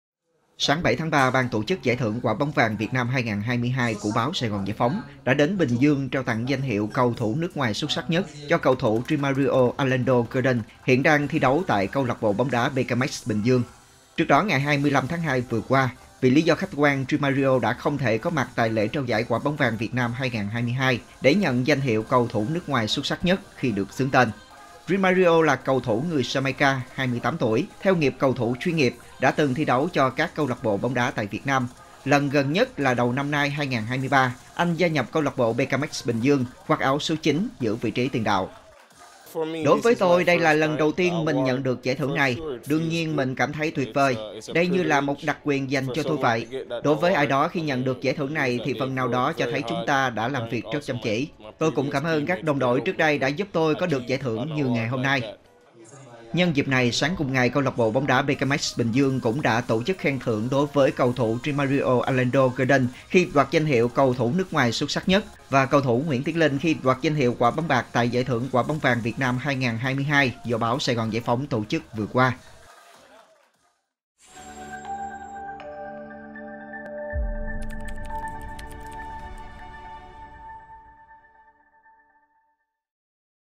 Báo SGGP trao danh hiệu “Cầu thủ nước ngoài xuất sắc nhất” cho Rimario
Sáng 7-3, Ban tổ chức Giải thưởng Quả bóng vàng Việt Nam 2022 của Báo SGGP đã đến Bình Dương trao tặng danh hiệu “Cầu thủ nước ngoài xuất sắc nhất” cho cầu thủ Rimario Allando Gordon hiện đang thi đấu tại Câu lạc bộ bóng đá Becamex Bình Dương.